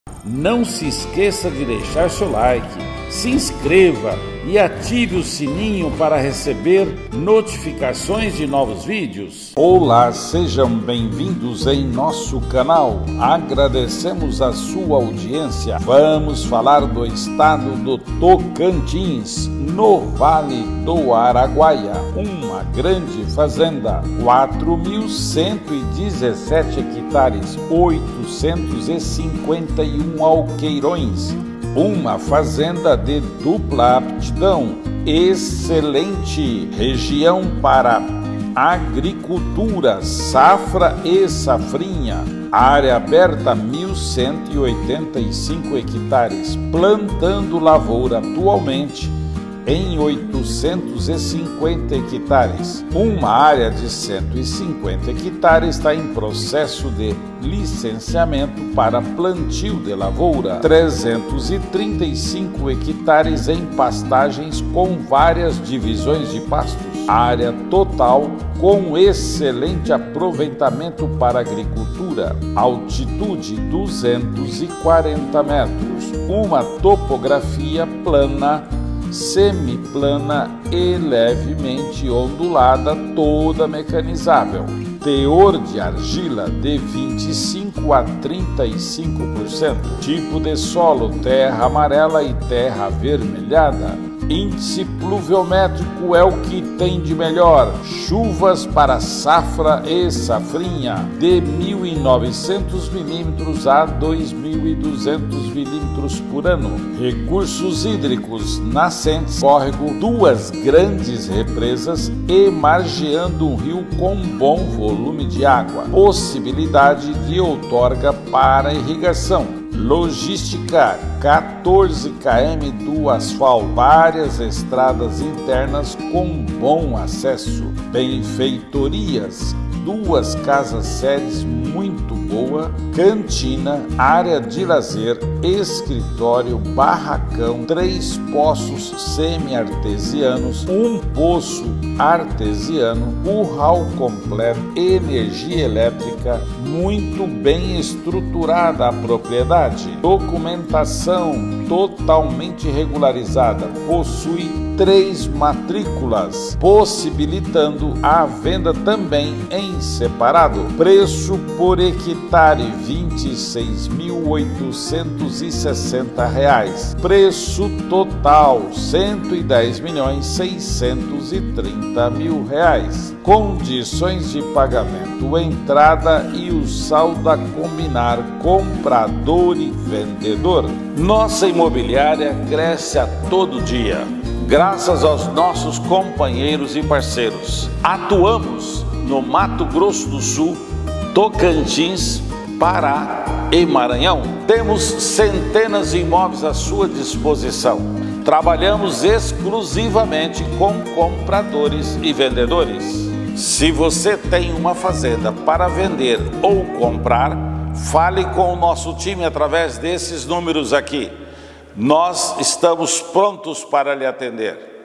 Fazenda plantando lavoura no Vale do Araguaia-TO 4.117,56 Hec., dupla aptidão, excelente logística
Localização: Vale do Araguaia, Tocantins 📍
Área total: 4.117,56 Hectares | 851 Alqueirões
Dupla Aptidão
Área Aberta: 1.185 hectares, plantando lavoura atualmente em 850 hectares, uma área de 150 hectares está em processo de licenciamento para plantio de lavoura,
335 hectares em pastagens com várias divisões de pastos. ✅
Área total com excelente aproveitamento para agricultura ✅
Altitude: 240 Metros
Topografia: Plana, semiplana e levemente ondulada
Teor de Argila: 25% à 35% ✅
Tipo de Solo: Terra amarela e terra avermelhada
Índice pluviométrico: 1.900 mm à 2.200 mm por ano 🌧
Recursos Hidrícos:
Nascentes, córrego,  02 grandes represas, e margeando um rio com bom volume de água
Possibilidade de outorga, para irrigação

Logística:
14 Km do asfalto,
Várias estradas internas com bom acesso. 🚚

Benfeitorias: 🏡
02 Casas sedes muito boa, cantina, Área de lazer,
Escritório, Barracão, 3 poços semi-artesianos, 1 poço artesiano,
Curral completo, energia elétrica, muito bem estruturada.

Documentação: Regularizada, Fazenda possuí 03 matriculas, possibilitando a venda separada. ✅
Preço por hectare: R$ 26.860,00 💵
Preço por Alqueirão: R$ 130.000,00 💵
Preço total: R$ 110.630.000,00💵
Condições de pagamento: Entrada, e saldo à combinar. ✅

⚠Valores sujeitos a alterações sem aviso prévio⚠

⚜Anuncie sua fazenda conosco, estamos em todas as plataformas digitais, Youtube, Instagram, Whatsapp, Facebook, LinkedIn, Olx, Zap, Viva real e Site.⚜